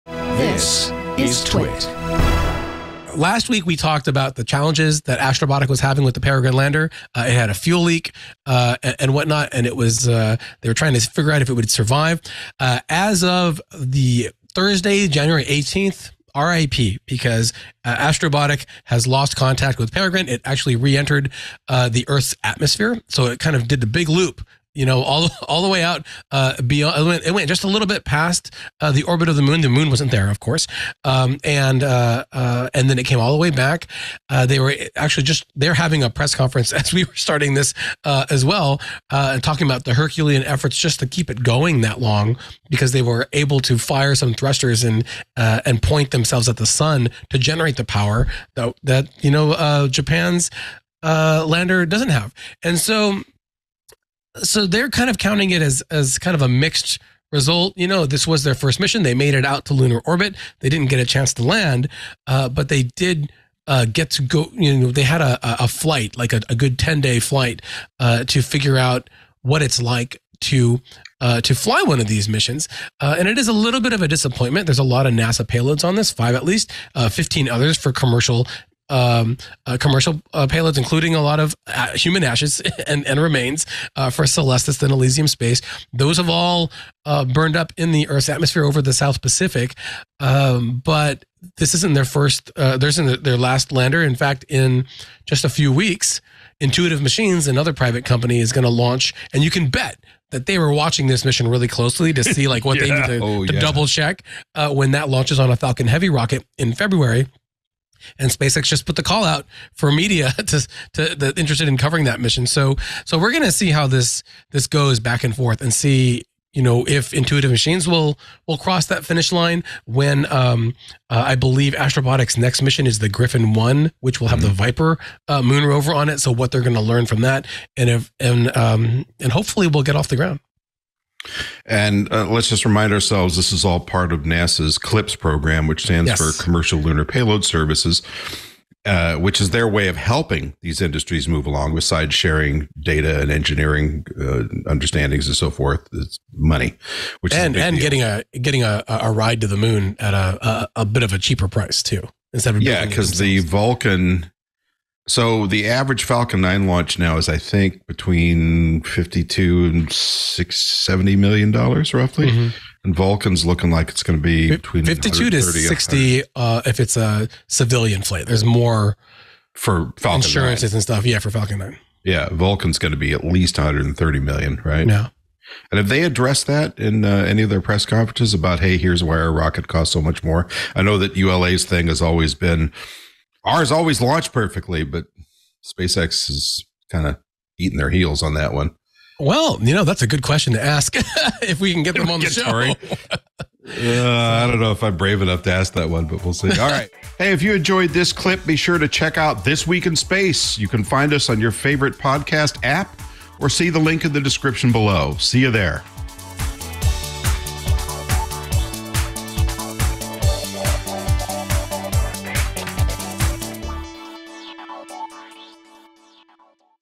Astrobotik's Peregrine Lander: From Launch to Loss
Uncover the dramatic journey and ultimate failure of Astrobotik's Peregrine Lander. Hosts Rod Pyle and Tariq Malik delve into the challenges faced, the launch details, and the crucial lessons learned from this mission. Want the full space exploration story? Tune into the complete episode!